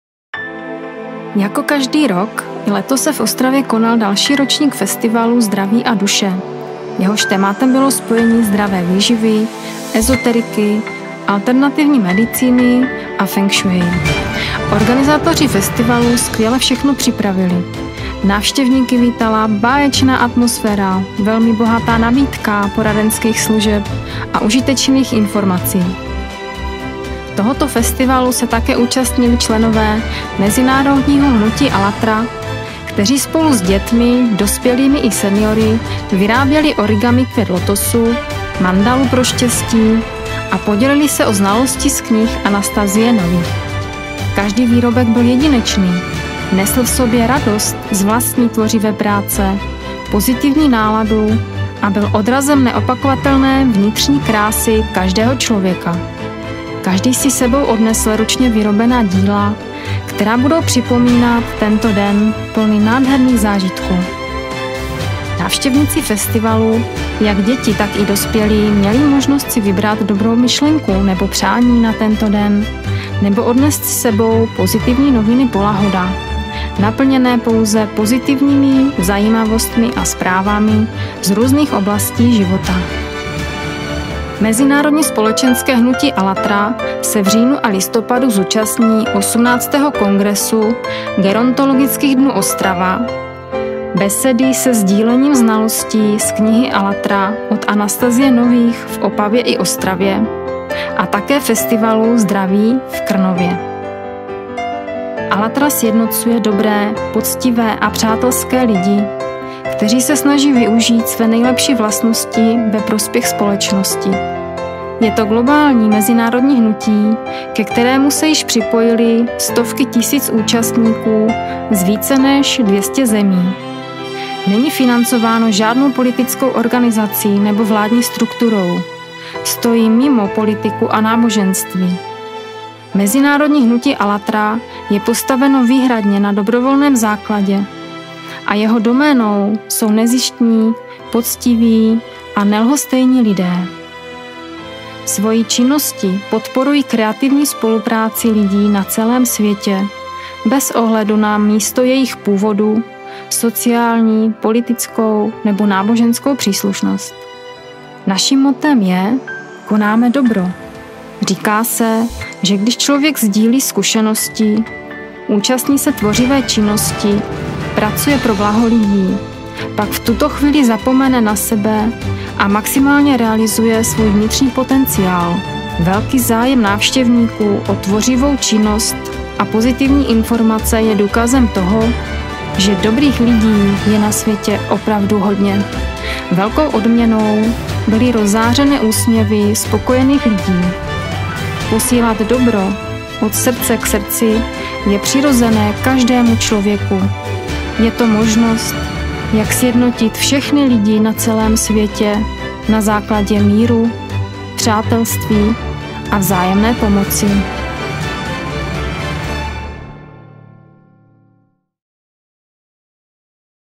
Výstava ZDRAVÍ a DUŠE 2014: dobrých lidí je na světě opravdu hodně!
Jako každý rok i letos se v Ostravě konal další ročník festivalu Zdraví a Duše, jehož tématem bylo spojení zdravé výživy, esoteriky, alternativní medicíny a feng-shui. Organizátoři festivalu skvěle všechno připravili. Návštěvníky vítala báječná atmosféra, velmi bohatá nabídka poradenských služeb a užitečných informací.
Říká se, že když člověk sdílí zkušenosti, účastní se tvořivé činnosti, pracuje pro blaho lidi, pak v tuto chvíli zapomene na sebe a maximálně realizuje svůj vnitřní potenciál. Velký zájem návštěvníků o tvořivou činnost a pozitivní informace je důkazem toho, že dobrých lidí je na světě opravdu hodně.
Velkou odměnou byly rozzářené úsměvy spokojených lidí. Posílat dobro od srdce k srdci je přirozené každému člověku, je to možnost, jak sjednotit všechny lidi na celém světě na základě míru, přátelství vzájemné pomoci.